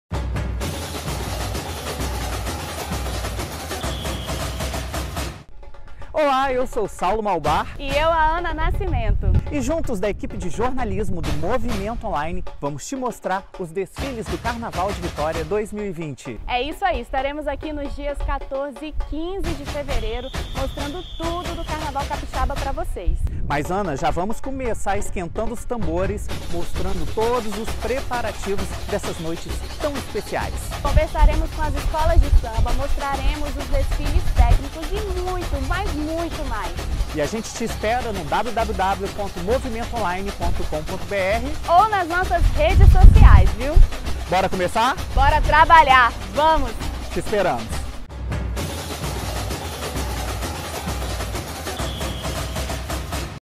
Carnaval 2020
🎊🎉O carnaval começa aqui!!!🎉🎊
O Sambão do Povo será palco dos desfiles das escolas de samba e o Movimento Online não ficará de fora da cobertura desta festa linda. Quer acompanhar o carnaval de Vitória? Então vem com a gente!